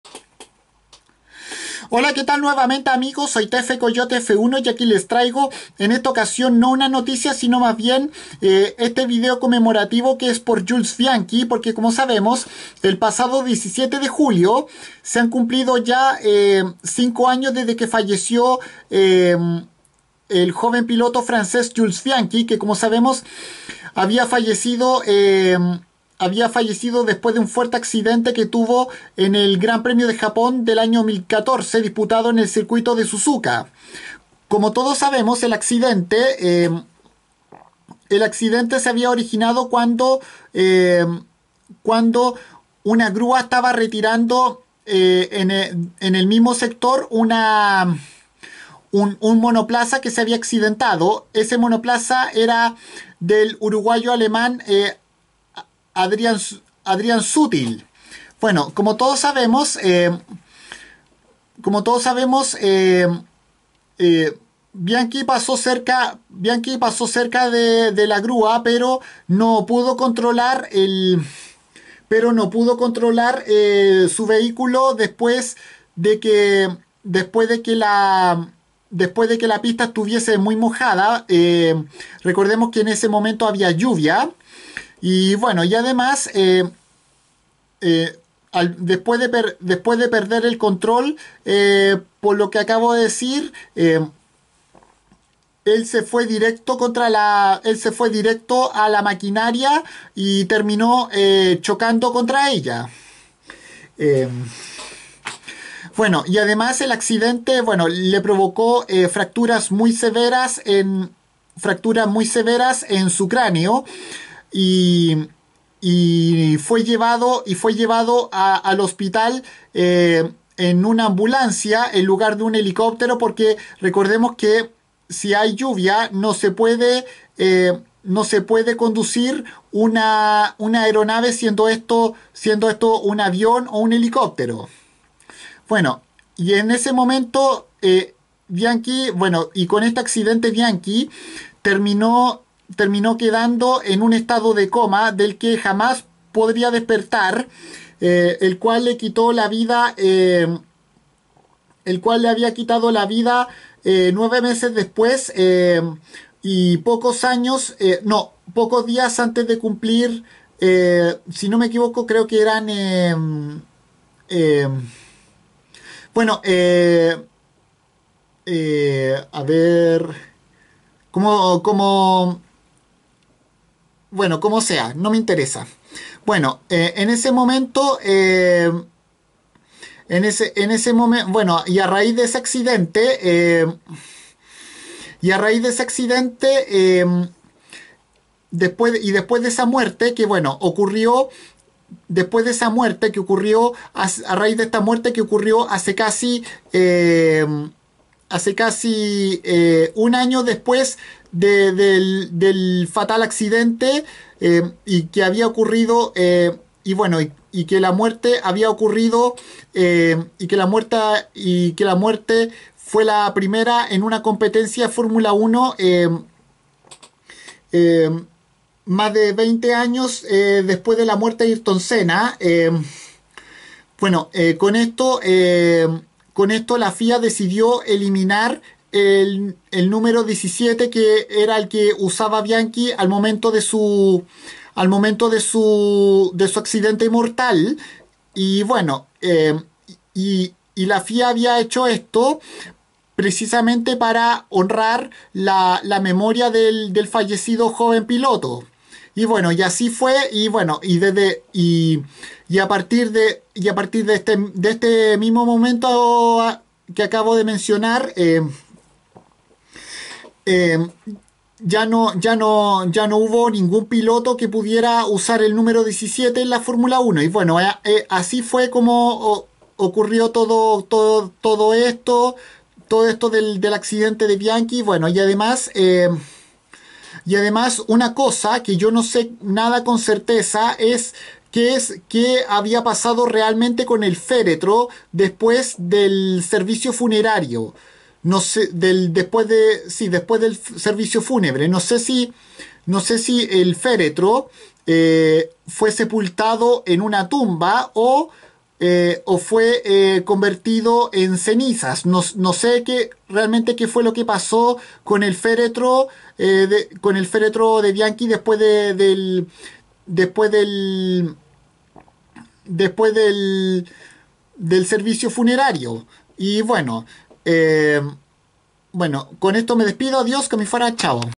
5 años después de la muerte de Jules Bianchi
el pasado 17 de Julio de este 2020 transcurrieron 5 años desde que el joven corredor francés perdiera la vida en 2015 casi 1 año después de un incidente en el GP de Japón de la Temporada 2014 siendo esta también la primera vez desde 1994 que falleciera un piloto en acto de servicio deportivo siendo este el brasilero triple campeón mundial Ayrton Senna Da Silva en el GP de Imola de dicha temporada